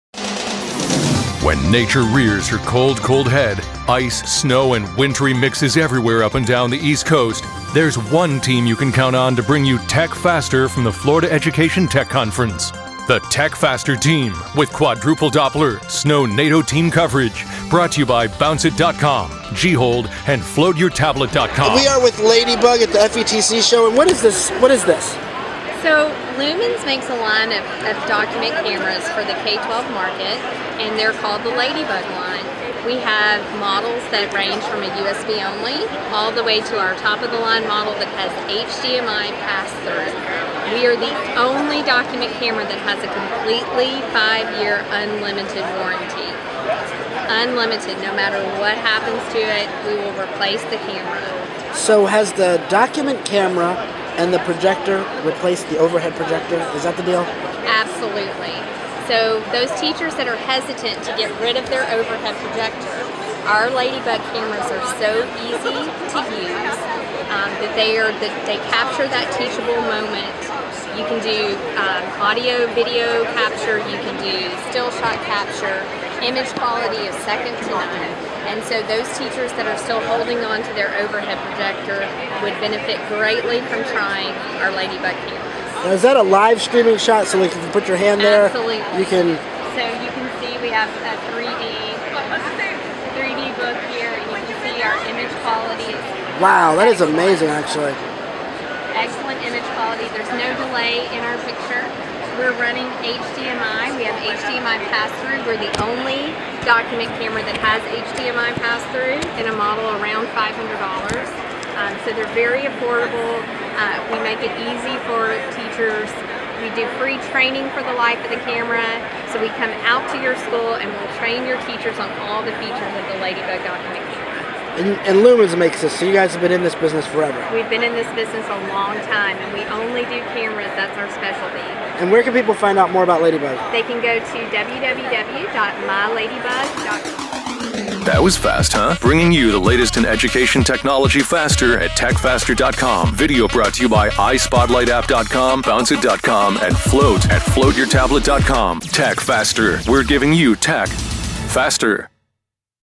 Ladibug: A New Take on the Overhead Projector [FETC 2014]
The Ladibug is incredibly interesting, and really a modern day overhead projector. It works in very much the same way as an overhead projector, but through a digital projector.

Essentially, you plug the Ladibug into a computer, and transmit the video captured from the Ladibug through a digital projector.